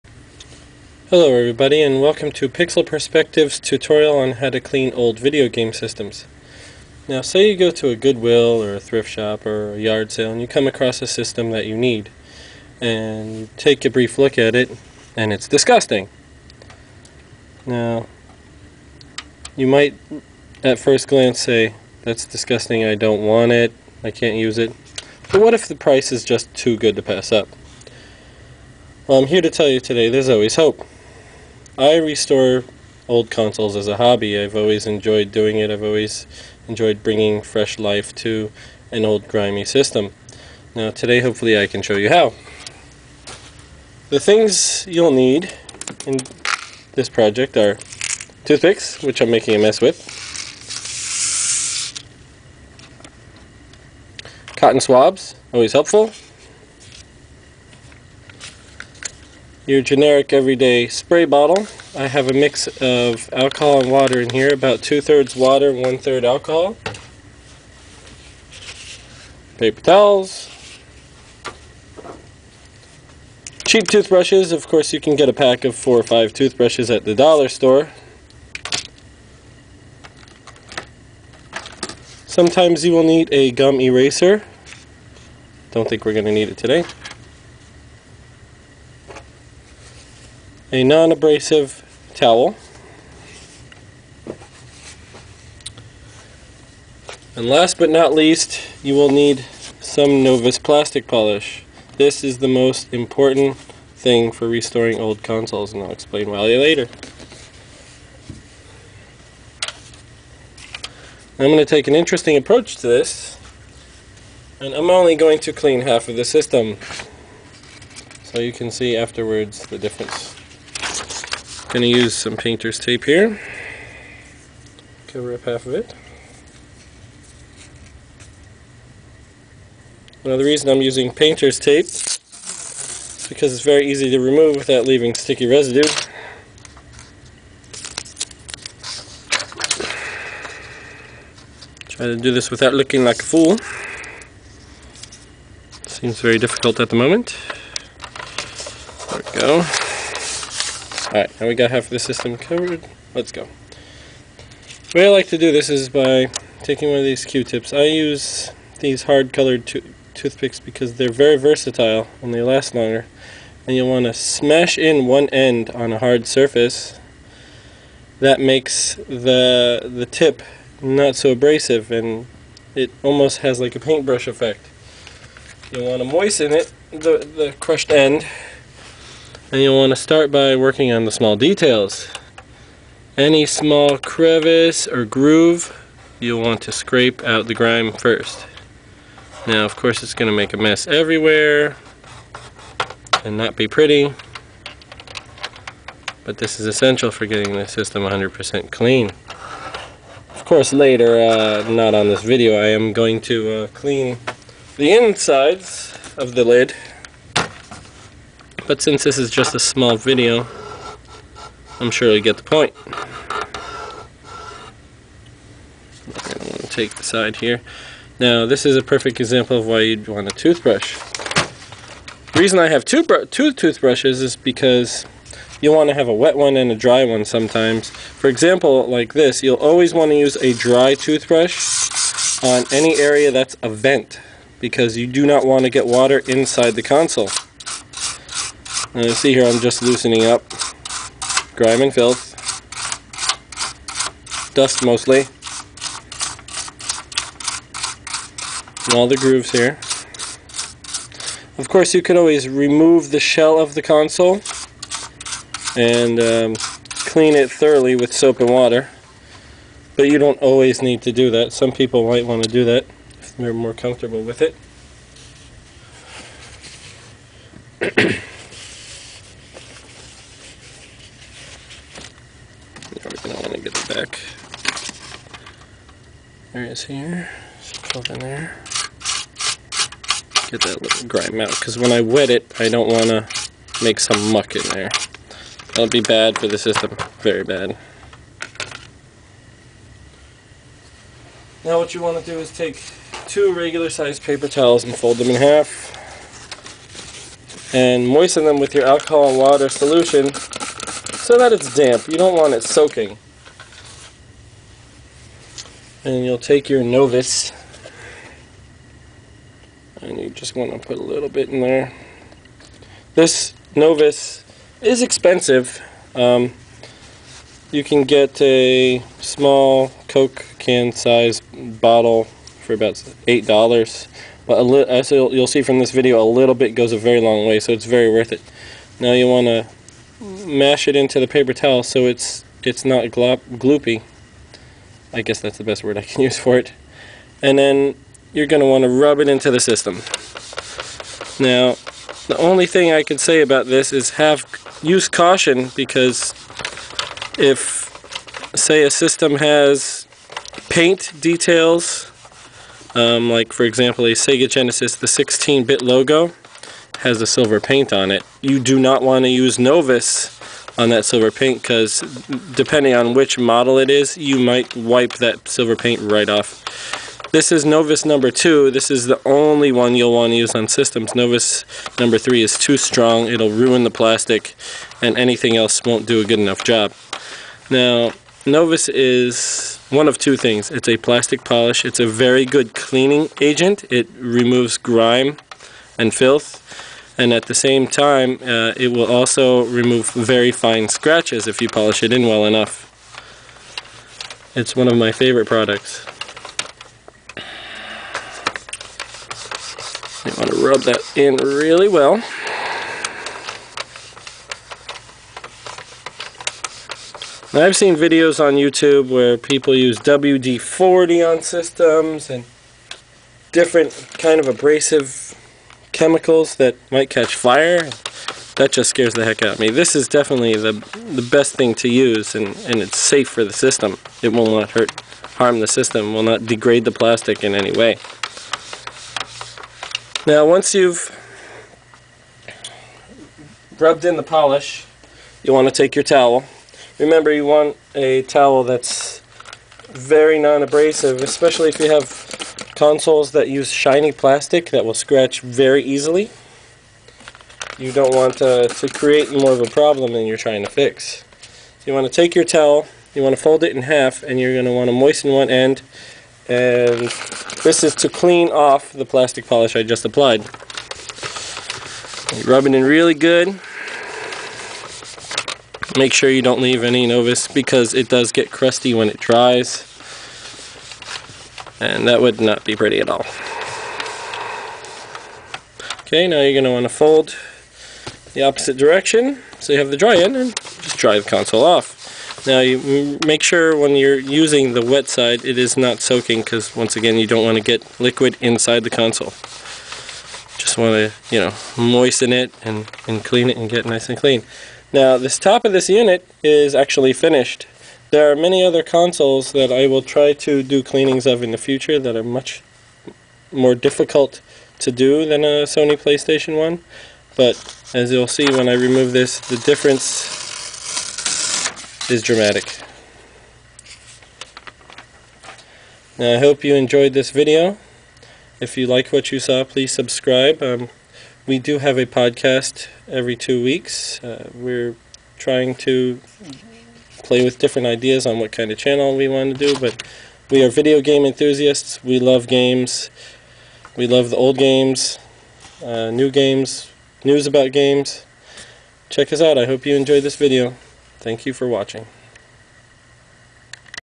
Retro Video Game Console Restoration and Cleaning Tutorial Sony PlayStation
George shows you how to restore a vintage console that you might usually pass up if you found it in the wild.